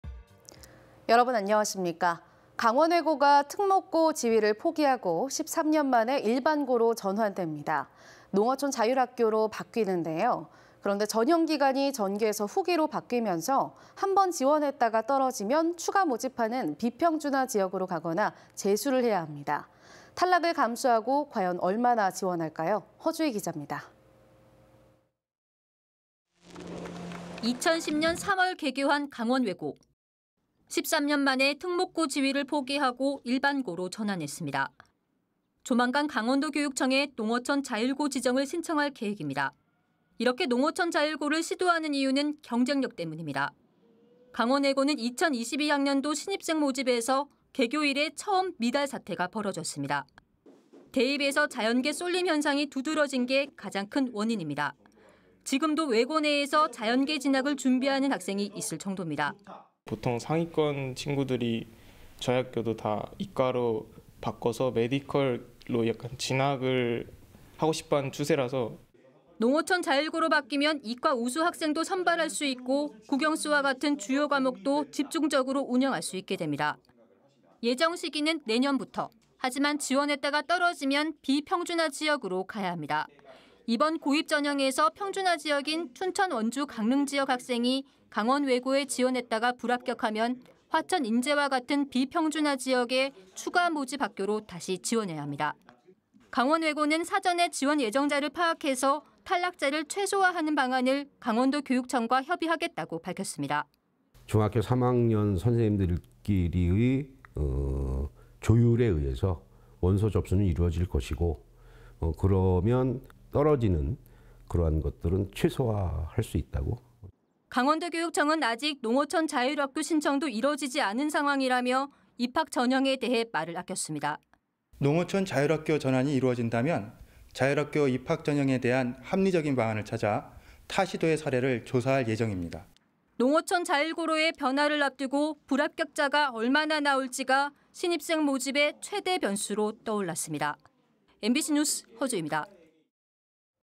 '일반고' 된 강원외고, 불합격하면?
강원외고가 특목고 지위를 포기하고
13년 만에 일반고로 전환됩니다.

 농어촌 자율학교로 바뀌는데요,

 그런데 전형기간이
전기에서 후기로 바뀌면서
한 번 지원했다가 떨어지면
추가 모집하는 비평준화 지역으로 가거나
재수를 해야 합니다.

 탈락을 감수하고 과연 얼마나 지원할까요?

 허주희 기자입니다.

           ◀ＶＣＲ▶

 2010년 3월 개교한 강원외고.

 13년 만에 특목고 지위를 포기하고
일반고로 전환했습니다.

 조만간 강원도교육청에
농어촌 자율고 지정을 신청할 계획입니다.

 이렇게 농어촌 자율고를 시도하는 이유는
경쟁력 때문입니다.

 강원외고는 2022학년도 신입생 모집에서
개교 이래 처음 미달 사태가 벌어졌습니다.

 대입에서 자연계 쏠림 현상이
두드러진 게 가장 큰 원인입니다.

 지금도 외고 내에서
자연계 진학을 준비하는 학생이
있을 정도입니다.

 ◀ＩＮＴ▶ 정시훈 /강원외고 3학년
"보통 상위권 친구들이, 저희 학교도 다 이과로 바꿔서
의대로 진학을 하고 싶어하는 추세라서..."

 농어촌 자율고로 바뀌면
이과 우수 학생도 선발할 수 있고
국·영·수와 같은 주요 과목도
집중적으로 운영할 수 있게 됩니다.

 예정 시기는 내년부터.

하지만 지원했다가 떨어지면
비평준화 지역으로 가야 합니다.

이번 고입 전형에서
평준화지역인 춘천, 원주, 강릉지역 학생이
강원외고에 지원했다가 불합격하면
화천, 인제와 같은 비평준화 지역의
추가 모집 학교로 다시 지원해야 합니다.

 강원외고는 사전에 지원 예정자를 파악해서
탈락자를 최소화하는 방안을
강원도교육청과 협의하겠다고 밝혔습니다.

 ◀ＩＮＴ▶ 주원섭 /강원외고 교장
"중학교 3학년 교사들끼리의 조율에 의해서
원서 접수는 이뤄질 것이고,
그러면 떨어지는 것은 최소화할 수 있다..."

 강원도교육청은 아직 농어촌 자율학교
신청도 이뤄지지 않은 상황이라며,
입학 전형에 대해 말을 아꼈습니다.

 ◀ＩＮＴ▶ 박세민 /강원도교육청 대변인
"농어촌 자율학교 전환이 이루어진다면
자율학교 입학전형에 대한 합리적인 방안을 찾아
타 시·도의 사례를 조사할 예정입니다."

 농어촌 자율고로의 변화를 앞두고
불합격자가 얼마나 나올 지가
신입생 모집의 최대 변수로 떠올랐습니다.

 MBC뉴스 허주희입니다.

 (영상취재: 김유완 / 그래픽: 전진호)

           ◀ＥＮＤ▶